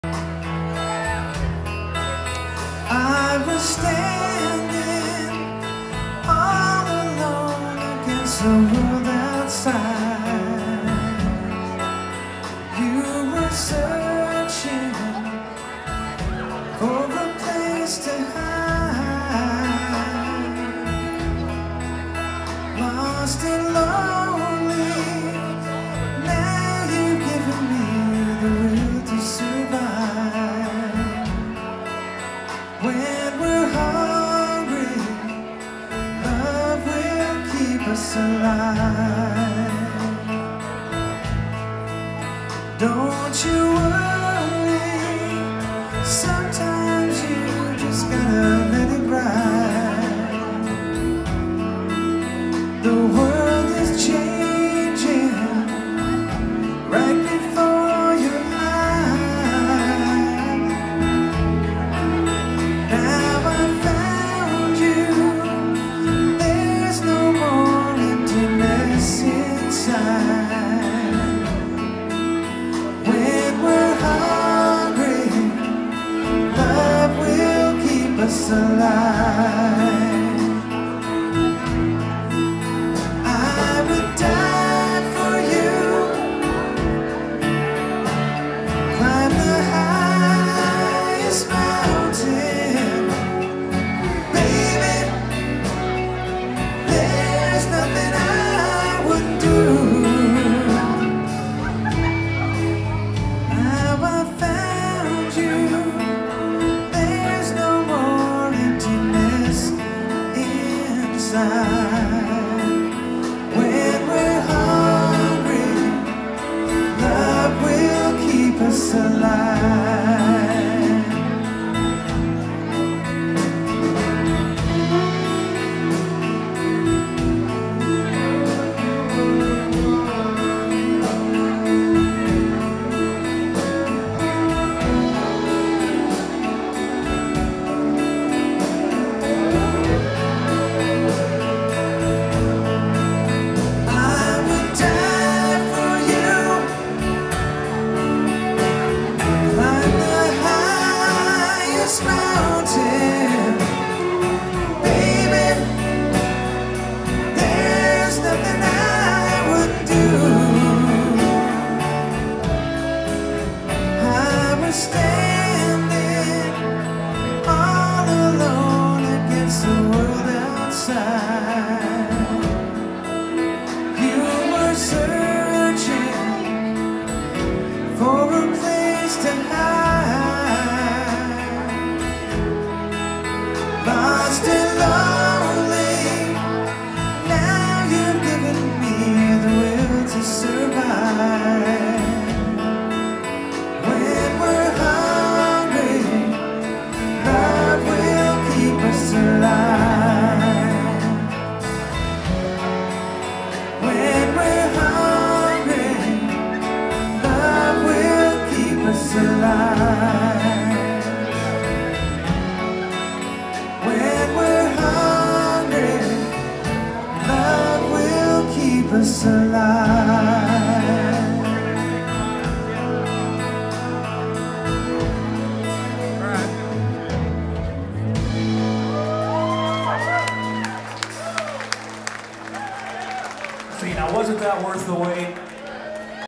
The Kings of Hollywood July 24, 2016 4
The Kings of Hollywood, Tribute to the Eagles (Arny Bailey, Randy Casey, Marc Montagnino, Norm Peterson, Martin Rush, Karl Haug) at the Canoes Cabaret inside the Tulalip Resort Casino, Tulalip WA. This Everett Rock Video produced in 4K.